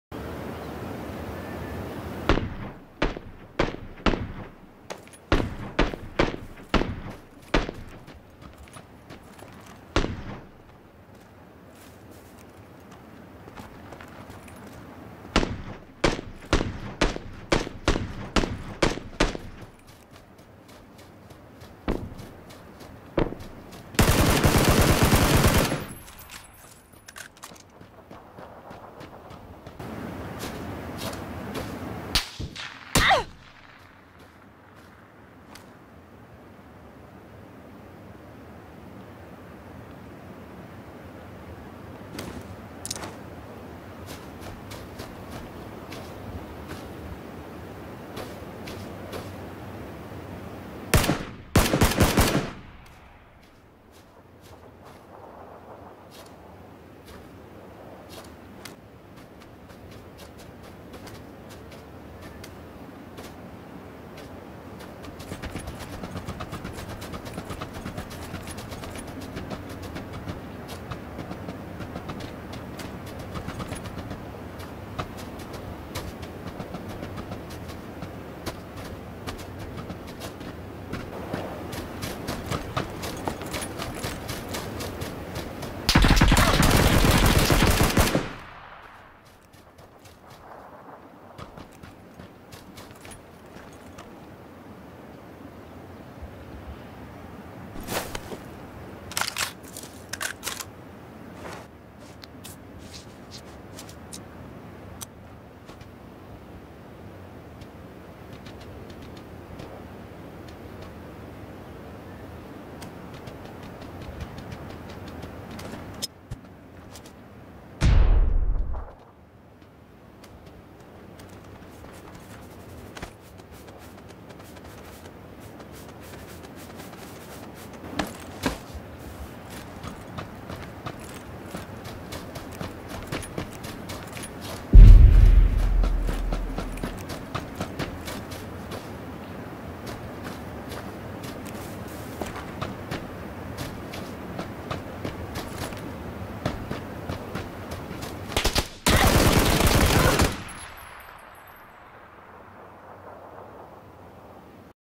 Solo FPP 1st win of v1.0
not too amazing or anything, but first solo win of 1.0!